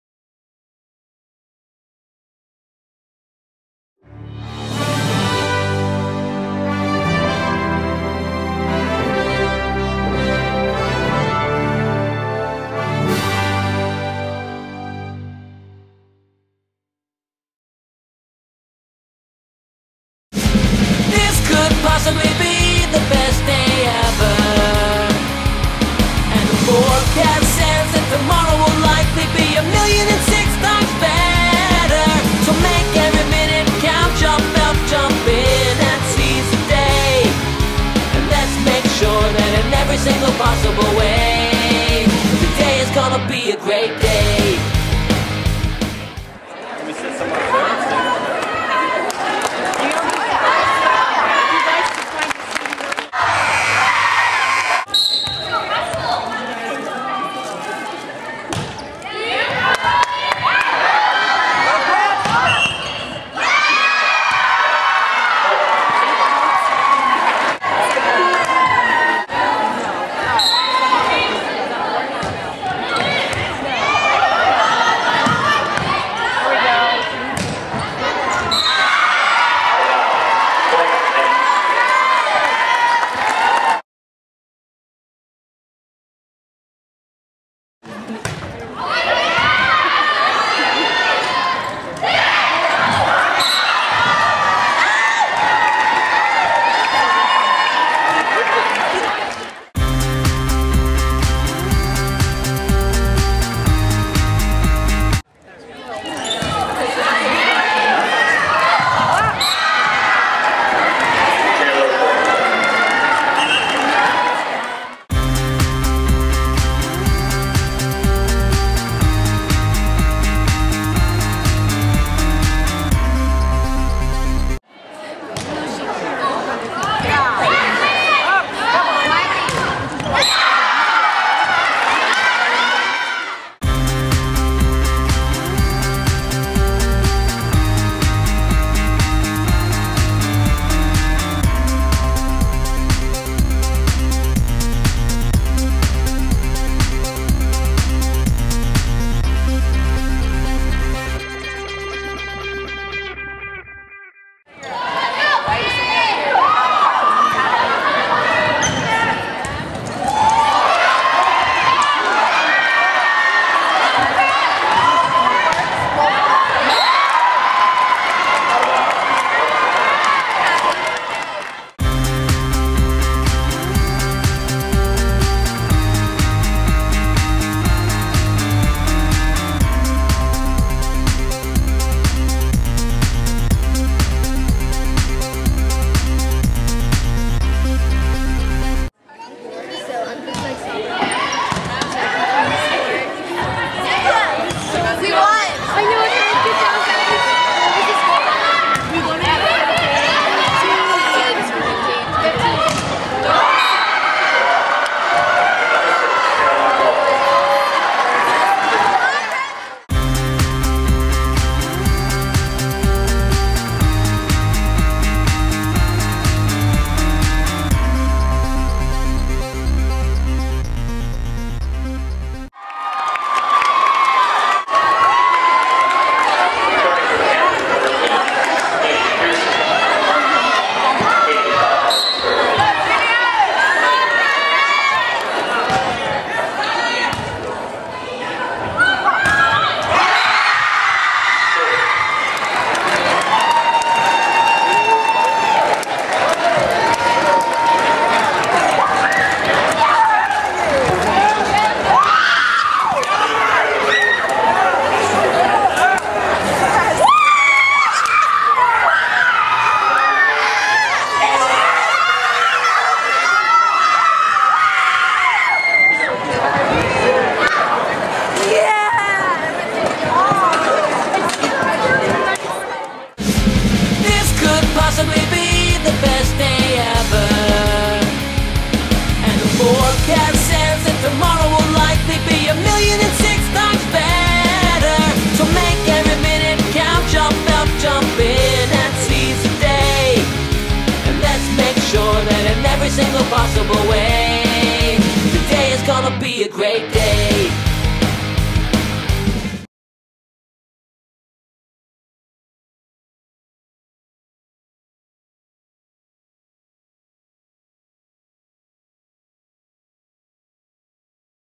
Girls Varsity Volleyball- St. Francis vs. Presentation
Sample preview from St. Francis High School vs. Presentation- Girls Varsity Volleyball game on 10/27/10. If you are interested in buying the full deluxe, extended version DVD, contact SideoutVideos.com